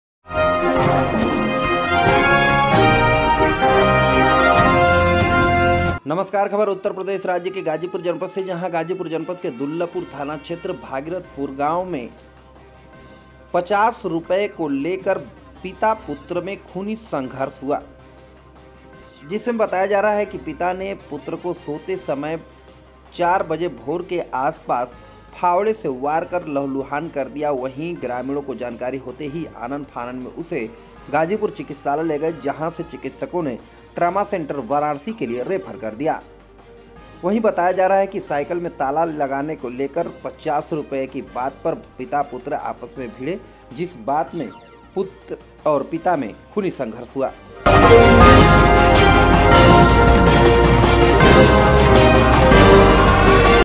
50 रुपये को लेकर बाप-बेटे मे खूनी संघर्ष#breakingnews बाप नें बेटे को फावड़े से काटाnews
*50 रुपये को लेकर बाप-बेटे में खूनी संघर्ष, बाप नें बेटे को फावड़े से काटा*
 दुल्लहपुर-गाजीपुर,स्थानीय थाना क्षेत्र के भागीरथपुर गाँव में साइकिल में ताला लगवाने के लिए पिता ने पुत्र को दिया था लेकिन बेटा नहीं लगवाया जिस बात को लेकर दोनों में शाम को कहा सुनी के बाद मारपीट हुई जिसमे बेटे नें बाप को बुरी तरह मारा पीटा फिर दोनों खाना खा कर सो गई फिर भोर में करीब 4 बजे के आसपास पिता नें सो रहे बेटे पर फावड़े से लगातार कई वार कर दिया जिससे उसको गम्भीर चोट आई है जिससे फस्ट ट्रिटमेंट के बाद जिलाअस्पताल से चिकित्सकों नें ट्रामा सेन्टर के लिए रेफर कर दिया ।
      प्राप्त जानकारी के अनुसार मोती राम अपने पुत्र रामप्रवेश उर्फ कल्लू को साइकिल में ताला लगवाने के लिए पचास रुपये दिए थे जिसे कई दिन बाद भी बेटे कल्लू नें नहीं लगवाया जब बाप बेटे शाम को जलालाबाद मार्केट में एक जगह हुवे तो गाली देते हुवे पिता नें बेटे से पूछ दिया कि ताला क्यो नहीं लगवाए हो जिनका बीच बाजार में पूछना बेटे को नागवार लगा उसी बात को लेकर बृहस्पति देर शाम घर आते ही पिता पुत्र आपस में भीड़ गए जबतक लोग कुच्छ समझ पाते दोनों के बीच मार पीट होने लगी बेटे नें नशे के धुत में पिता को बुरी तरह मारा पीटा   जिसे देख आसपास के लोगों ने समझा बुझा कर मामला शांत करवाया फिर दोनों खाना खा कर सो गए फिर अचानक शुक्रवार की सुबह करीब 4 बजे के आसपास पिता नें सो रहे बेटे पर फावड़ा से लगातार कई वार कर दिया जिससे बेटे रामप्रवेश उर्फ कल्लू को सर,गर्दन,कन्धा सहित कई जगह गम्भीर चोट लग गई चीख पुकार सुनते ही आसपास के लोग इकट्ठा हो गए खून से लतफत रामप्रवेश की हालत गम्भीर देख आनन फानन में उसे लोग जिलाअस्पताल गाजीपुर ले गए जहाँ से फस्ट ट्रिटमेंट के बाद चिकित्सकों नें ट्रामा सेन्टर वाराणसी के लिए रेफर कर दिया ग्रामीणों नें बताया कि पिता मजदूरी करता था जिनके तीन पुत्र है बड़ा बेटा अवधू व सबसे छोटा पंकज गुजरात रहकर कमाते हैं रामप्रवेश दूसरे नम्बर पर था जो घर पर ही रहकर राजमिस्त्री का काम करता है वहीं घटना की जानकारी ग्रामीणों द्वारा मिलते ही पुलिस जाँच में जुट गई जिस सम्बन्ध में चौकी प्रभारी जलालाबाद रितेश द्विवेदी ने बताया कि ग्रामीणों द्वारा जानकारी मिली है जिस घटना की हर एंगल से जाँच की जा रही है मामला पिता-पुत्र का होने की वजह से कोई तहरीर नहीं मिली है जांचोपरांत जो भी विधिक कार्यवाही होगी कि जाएगी ।